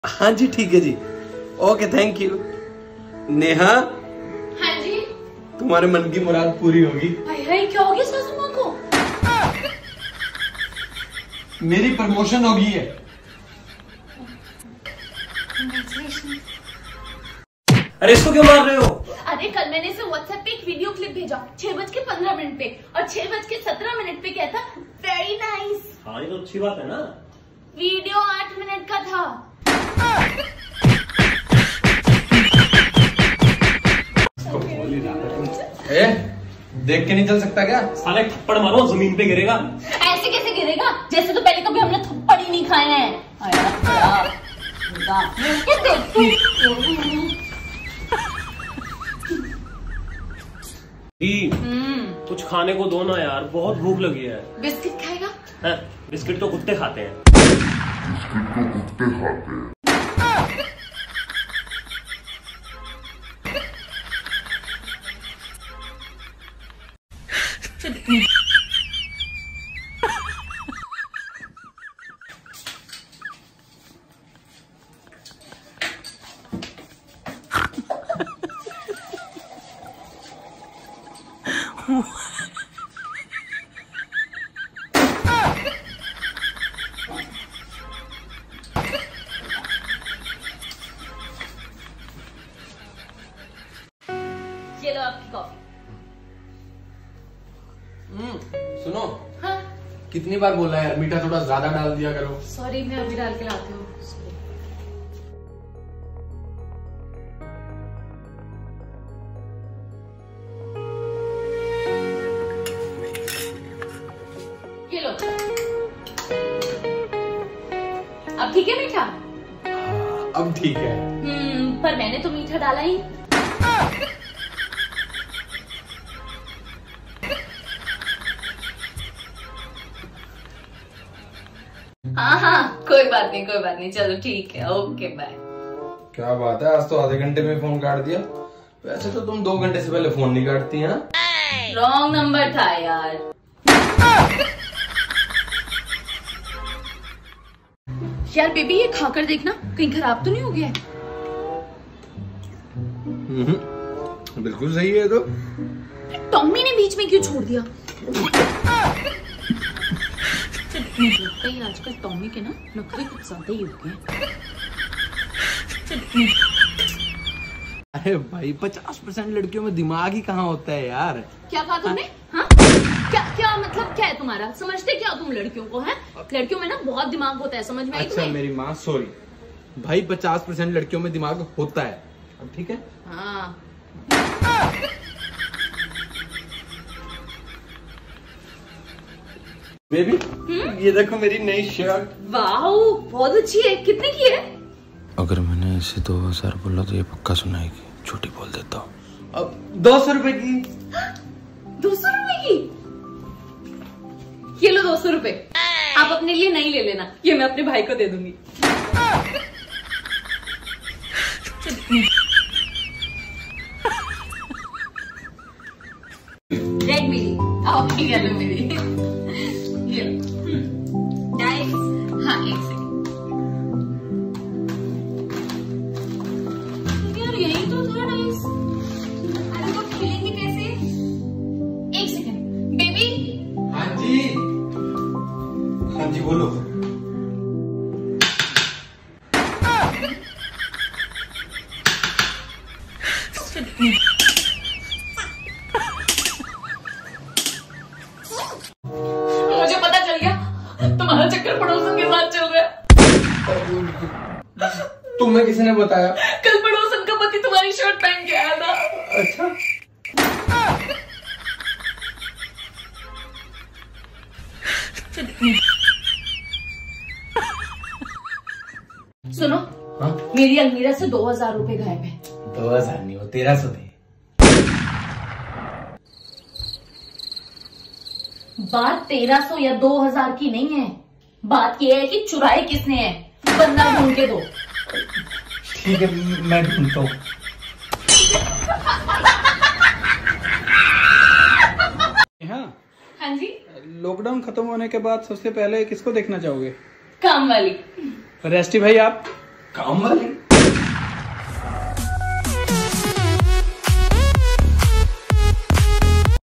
Funny videos of Resty Kamboj and Neha Bagga | Relationship | Comedy | Tiktok | NockJhok
Hope you all are doing good. This video is all about funny things in relationship.